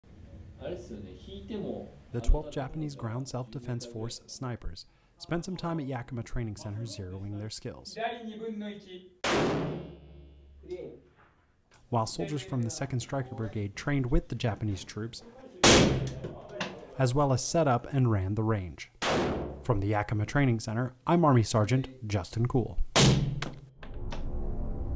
Japanese Self Defense Force Snipers Training
12th Brigade Japanese Ground Self Defense Force snipers train with 2-2 SBCT Snipers at Yakima Training Center.

YAKIMA TRAINING CENTER, WA, UNITED STATES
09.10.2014
Video by Staff Sgt. Justin Kuhl
5th Mobile Public Affairs Detachment